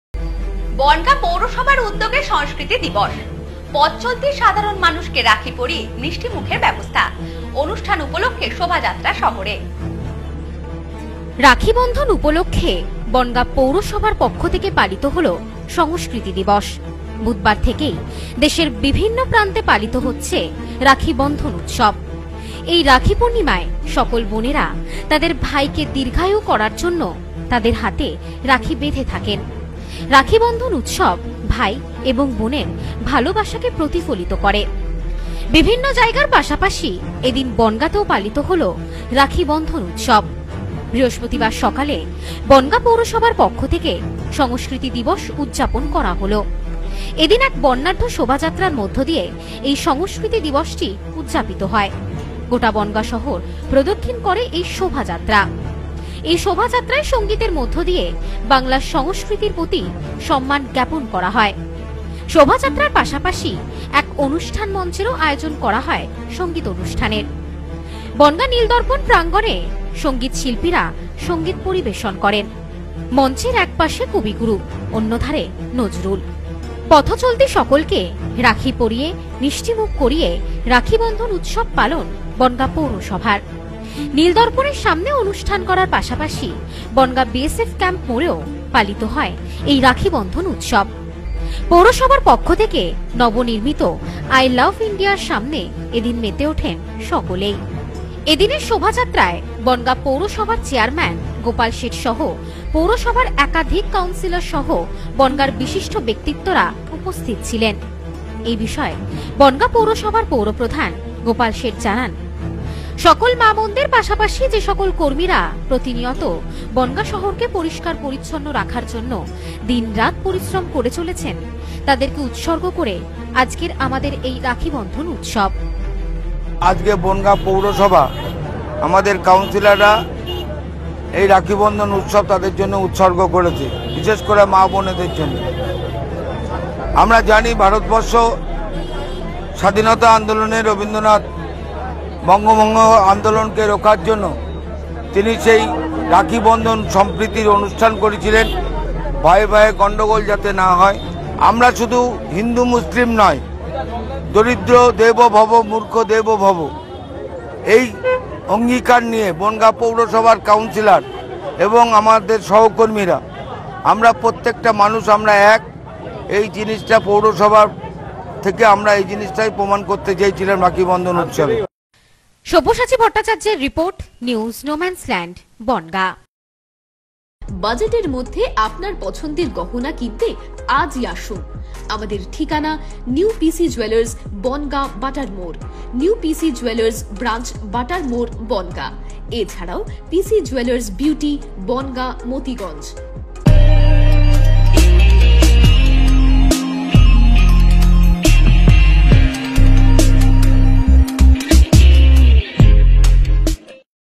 বনগাঁ পৌরসভার উদ্যোগে সংস্কৃতি দিবস।
বনগাঁ পৌরসভার উদ্যোগে সংস্কৃতি দিবস। পথ চলতি সকল কে রাখি পরিয়ে মিষ্টিমুখের ব্যবস্থা। অনুষ্ঠান উপলক্ষে শোভাযাত্রা শহরে।